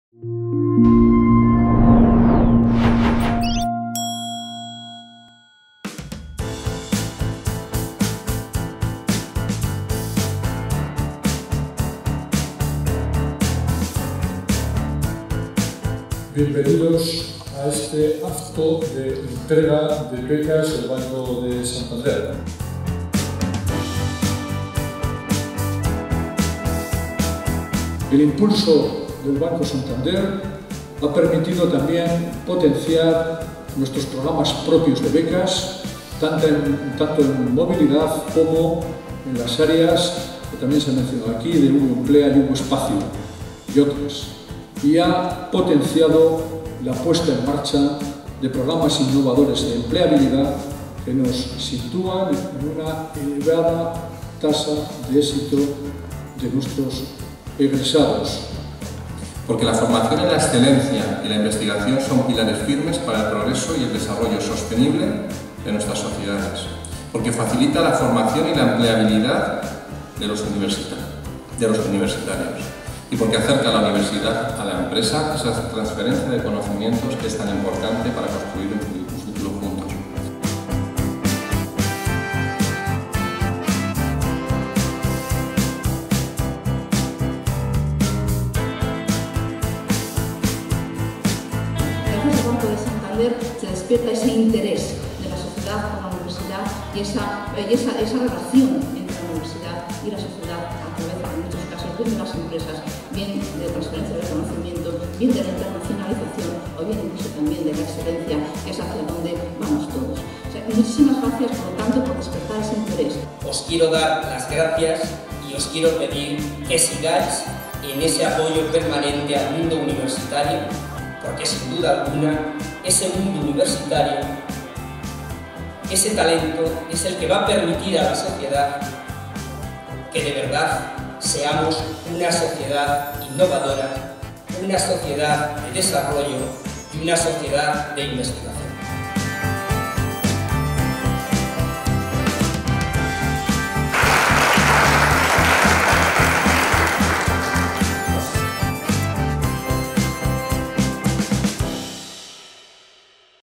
Actualidad UBU - Entrega de diplomas Banco Santander
El Rector de la Universidad de Burgos, Manuel Pérez Mateos, preside el acto de entrega de los diplomas y credenciales a los estudiantes que han participado en los programas de las Becas Santander. Estos programas incluyen las becas CRUE-CEPYME, becas internacionales y de movilidad con Iberoamérica y las becas propias concedidas a la UBU (Ubuespacio y televisión de la Universidad)

Durante la sesión intervendrá además del rector, el director territorial de Castilla y León del Banco Santander, Óscar Calleja Ruiz, así como algunos de los beneficiarios de los programas que compartirán sus experiencias.

En el acto se homenajeará al Santander Universidades y se hará un balance de la colaboración que ha existido entre ambas instituciones.

Estos programas -financiados íntegramente por el Banco de Santander a través de su División Global Santander Universidades- constituyen para los alumnos una extraordinaria oportunidad académica y personal.